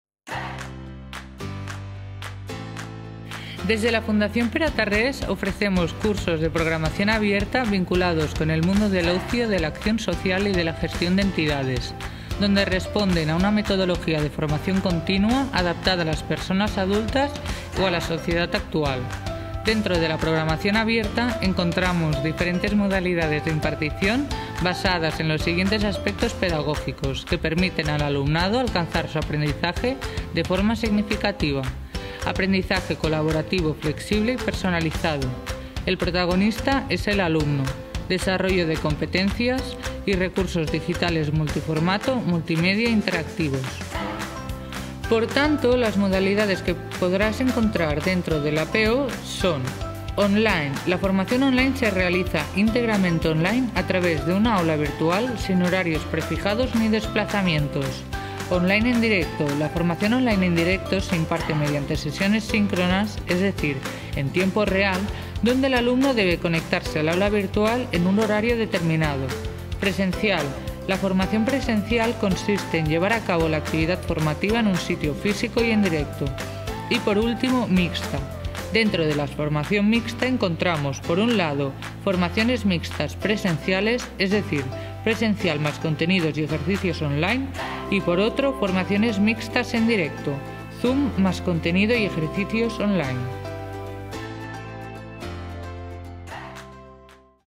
Modalidades de formación de los cursos online. Fundación Pere Tarrés
Desdel Centro de Formación Pere Tarrés ofrecemos cursos en diferentes áreas, modalidades y niveles. A través de este vídeo te explicamos las modalidades de formación de los cursos online.